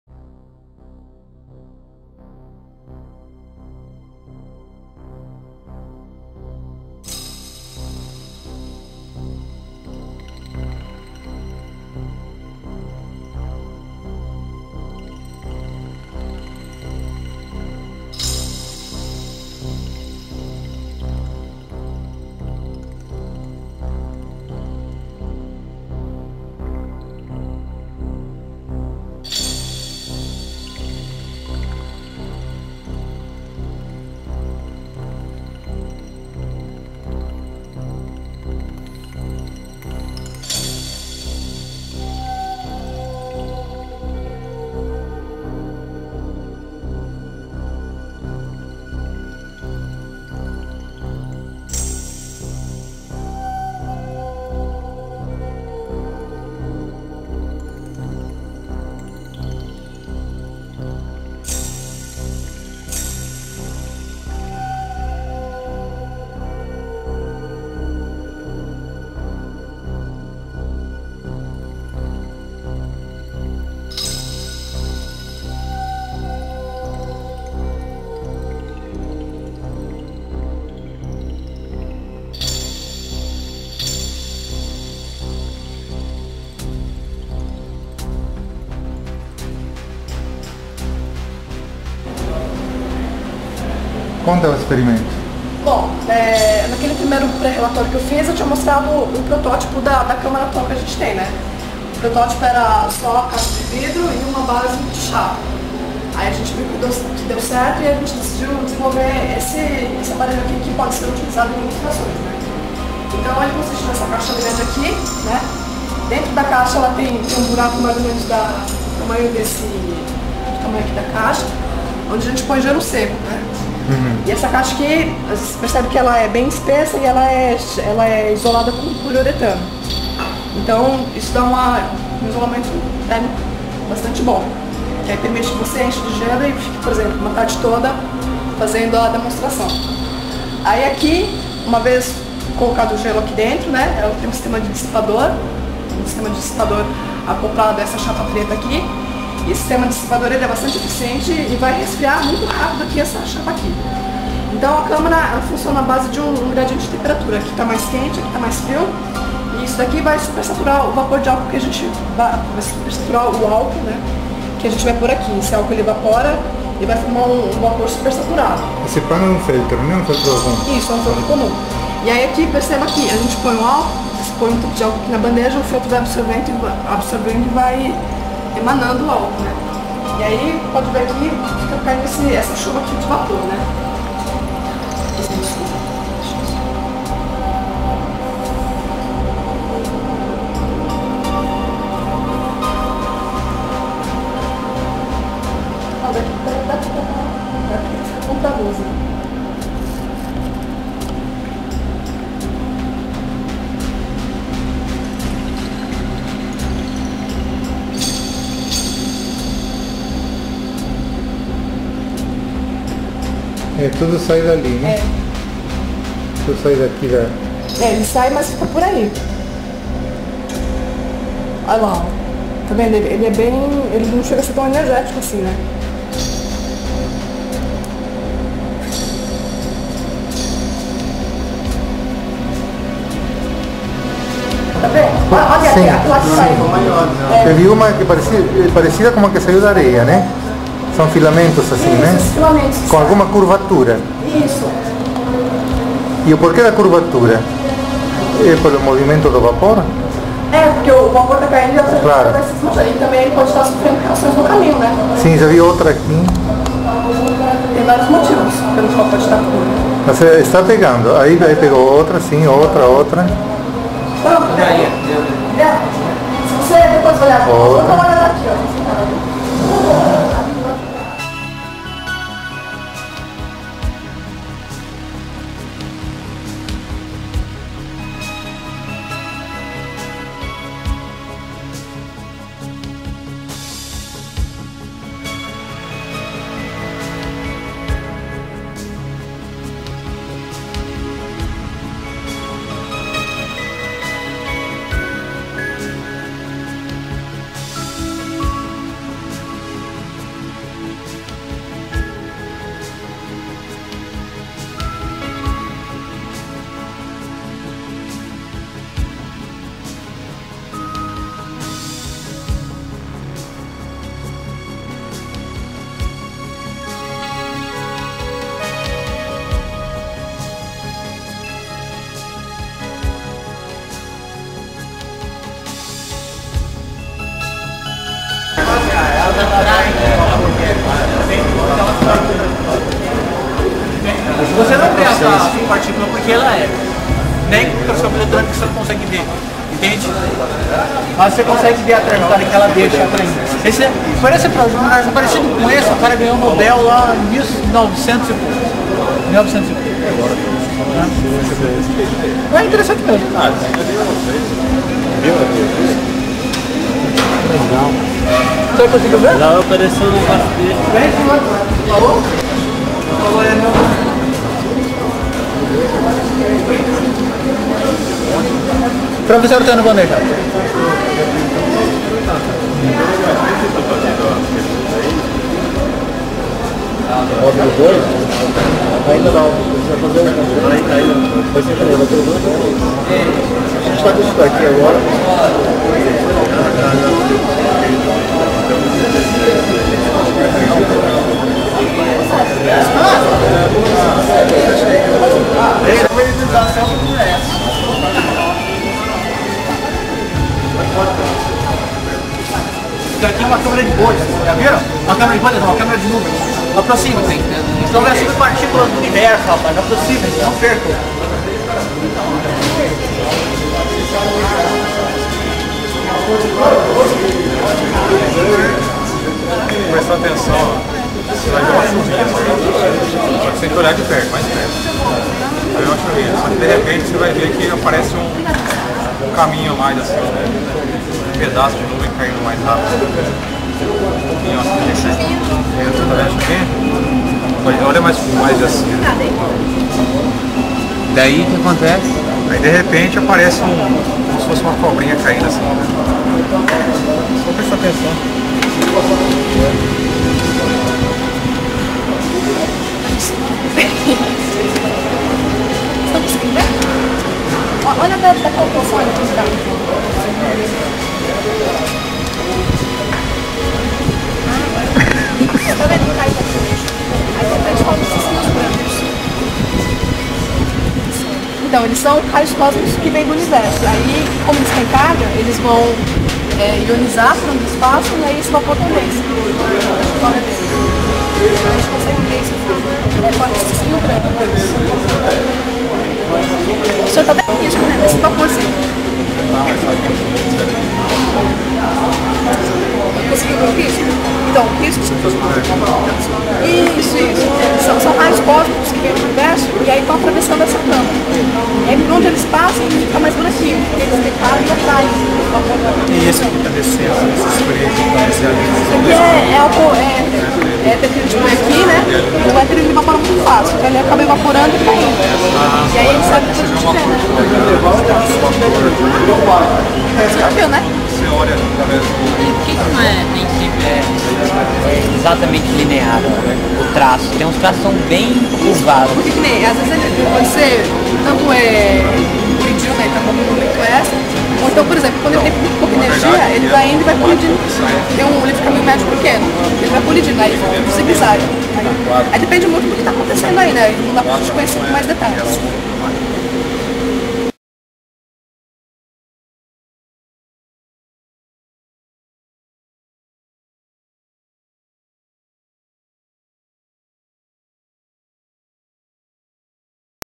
Câmara de nuvens para detetar partículas cósmicas
Trabalho de ensino realizado no Instituto de Física da UNICAMP mostrando ao público a passagem de partículas cósmicas e radiativas na forma de traços no vapor.